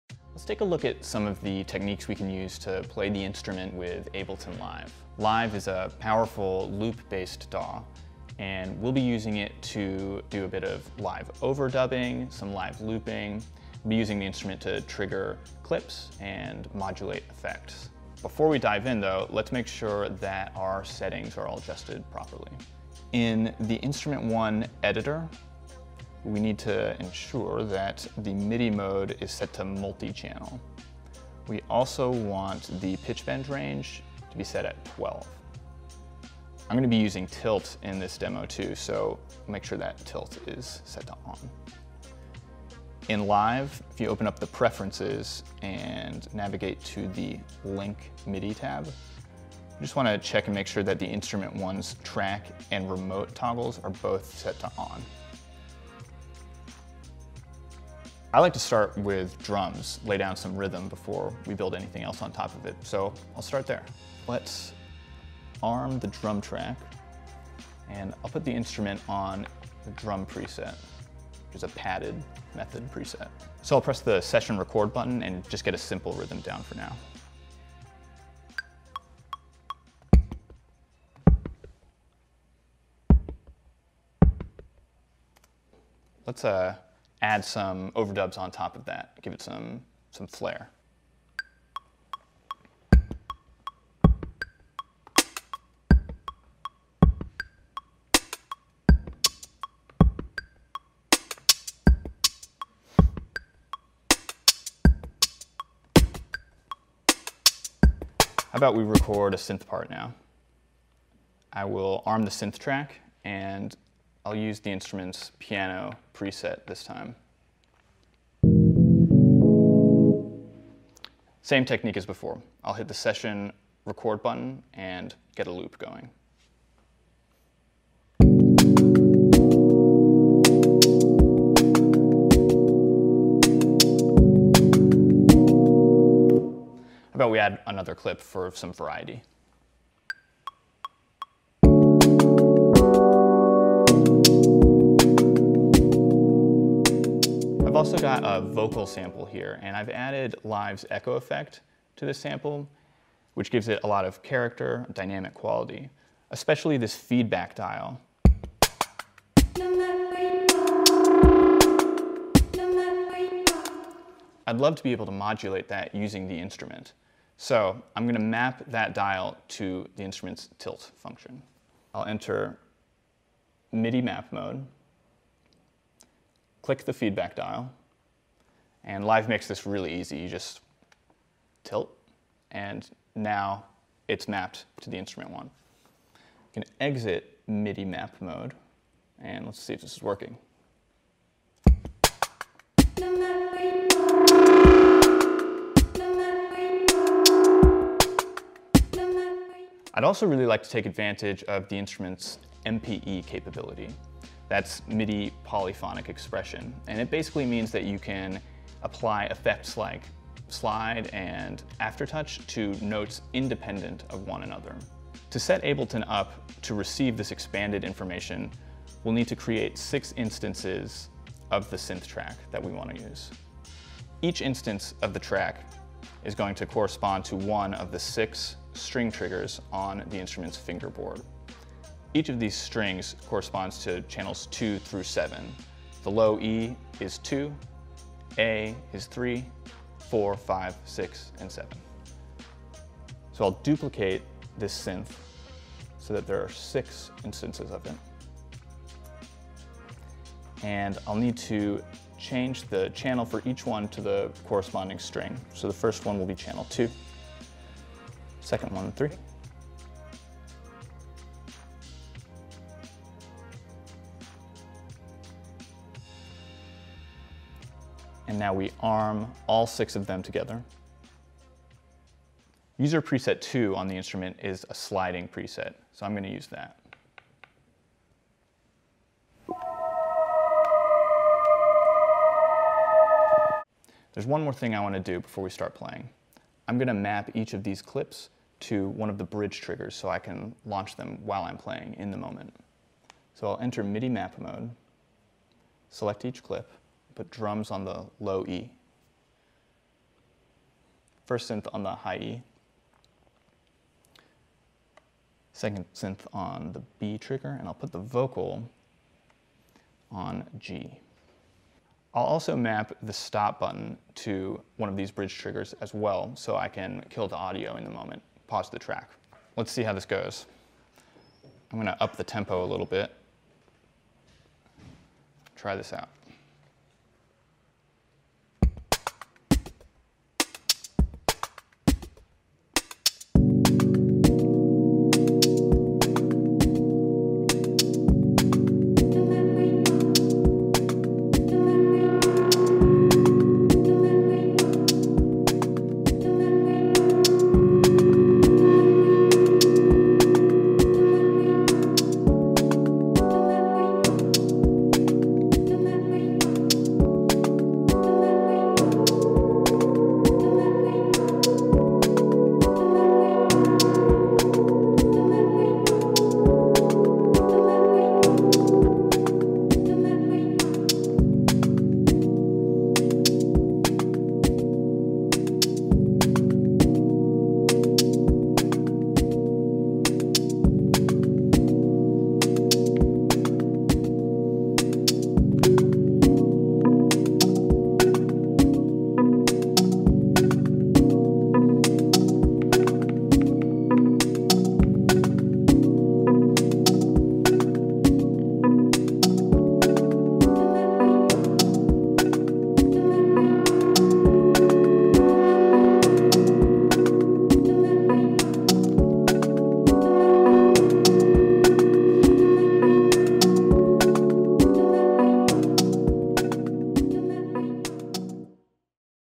Using The Artiphon INSTRUMENT 1 with Ableton Live 10
Here at Artiphon, we love Ableton, and use their digital audio workstation, Ableton Live, all the time. Because the Artiphon INSTRUMENT 1 is a USB MIDI controller, it plugs into workstations like Live right out of the box—no configuration needed. 📦 Let's take a look at a few ways you can incorporate the Artiphon INSTRUMENT 1 into your Ableton Live 10 workflow.

Transcript:

00:00
Let's take a look at some of the techniques we can use to play the instrument with Ableton Live. Live is a powerful, loop-based DAW, and we'll be using it to do a bit of live overdubbing, some live looping. We’ll be using the instrument to trigger clips and modulate effects. Before we dive in though, let's make sure that our settings are all adjusted properly.

00:23
In the INSTRUMENT 1 Editor, we need to ensure that the MIDI mode is set to multichannel. We also want the pitch bend range to be set at 12. I'm going to be using Tilt in this demo, too, so make sure that Tilt is set to ’On’. In Live, if you open up the preferences and navigate to the ‘Link/MIDI’ tab, you just want to check and make sure that the INSTRUMENT 1's ‘Track’ and ‘Remote’ toggles are both set to ‘On’.

00:58
I like to start with drums, lay down some rhythm before we build anything else on top of it, so I'll start there. Let's arm the drum track, and I'll put the instrument on the Drum preset. That’s a padded method preset. So, I'll press the Session Record button and just get a simple rhythm down for now.

01:26
Let's add some overdubs on top of that, give it some flare.

01:48
How about we record a synth part now? I will arm the synth track and I'll use the instrument's Piano preset this time. Same technique as before. I'll hit the Session Record button and get a loop going.

02:05
How about we add another clip for some variety? I've also got a vocal sample here, and I've added Live's Echo effect to the sample, which gives it a lot of character and dynamic quality, especially this feedback dial.

02:44
I'd love to be able to modulate that using the instrument, so I'm going to map that dial to the instrument's Tilt function. I'll enter MIDI Map Mode, click the Feedback dial, and Live makes this really easy. You just Tilt, and now it's mapped to the INSTRUMENT 1. You can exit MIDI Map Mode, and let's see if this is working.

03:10
I'd also really like to take advantage of the instrument's MPE capability. That's MIDI Polyphonic Expression, and it basically means that you can apply effects like slide and aftertouch to notes independent of one another. To set Ableton up to receive this expanded information, we'll need to create 6 instances of the synth track that we want to use. Each instance of the track is going to correspond to one of the six string triggers on the instruments fingerboard. Each of these strings corresponds to channels 2 through 7. The low E is 2, A is 3, 4, 5, 7 and 7. So, I'll duplicate this synth so that there are 6 instances of them. I'll need to change the channel for each one to the corresponding string. So, the first one will be channel 2, second one, 3, and now we arm all six of them together. User Preset two on the instrument is a sliding preset, so I'm going to use that.

04:44
There's one more thing I want to do before we start playing. I'm going to map each of these clips to one of the bridge triggers so I can launch them while I'm playing in the moment. So, I'll enter MIDI Map Mode, select each clip, put drums on the low E, first synth on the high E, second synth on the B trigger, and I'll put the vocal on G. I'll also map the ‘Stop’ button to one of these bridge triggers as well so I can kill the audio in the moment, pause the track. Let's see how this goes. I'm going to up the tempo a little bit. Let’s try this out.

Follow Artiphon on socials! Tag @Artiphon and OrbaLoops or INSTRUMENT1 to share your videos and get featured.